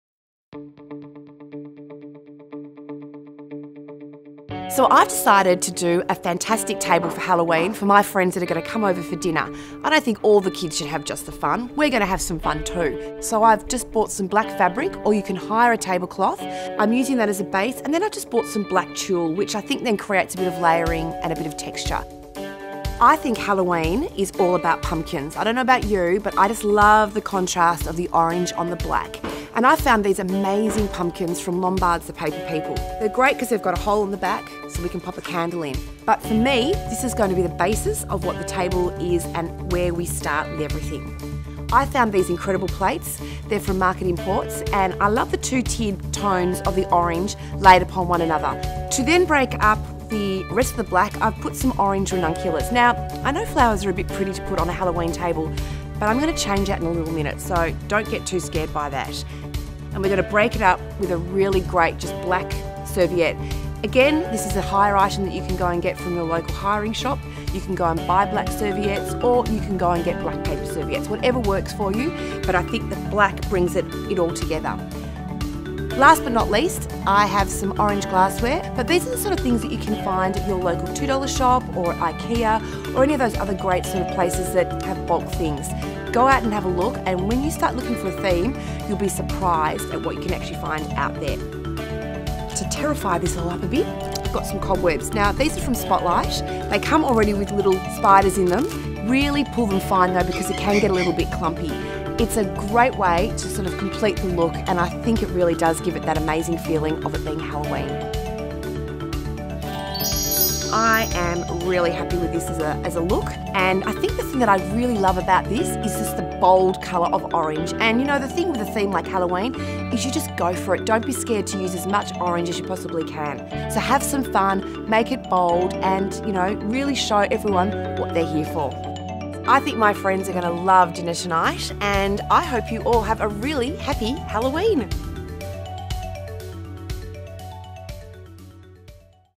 A Terrifyingly Good Halloween Dinner Table
Are you planning a Halloween dinner party this weekend? If so, you must watch this video - I've set and dressed a fun table for a Halloween dinner party which, If I may say, looks pretty cool! This look is so quick and easy to achieve with little cost. Happy Halloween!!